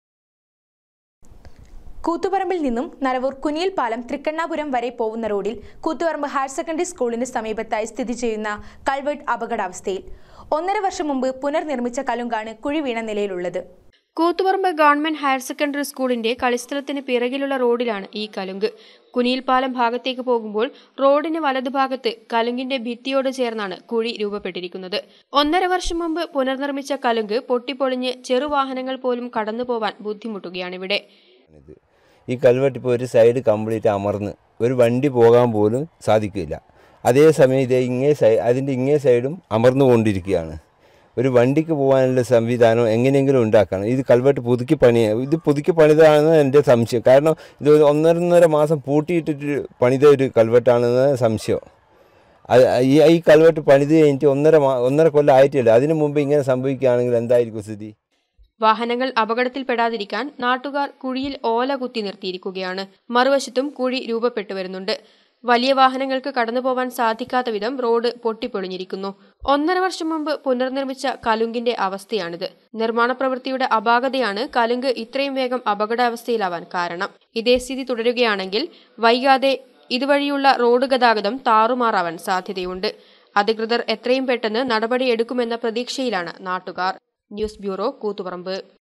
കൾവർട്ട് അപകടാവസ്ഥയിൽ
കൂത്തുപറമ്പ് നരവൂർ കുനിയിൽ പാലം റോഡിലുള്ള കൾവർട്ട് അപകടാവസ്ഥയിൽ.